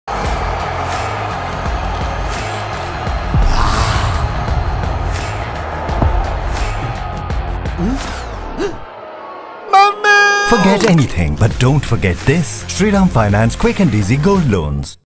Don't Forget About Quick & Easy Gold Loan | Flexible Repayment Tenure | Shriram Finance Gold Loan
Don't forget about this quick and easy finance option!
With low interest rates and flexible repayment options, Shriram Finance Gold Loan can be a great way to reach your financial goals.

Be rest assured that your gold jewellery is safe and handled with utmost care.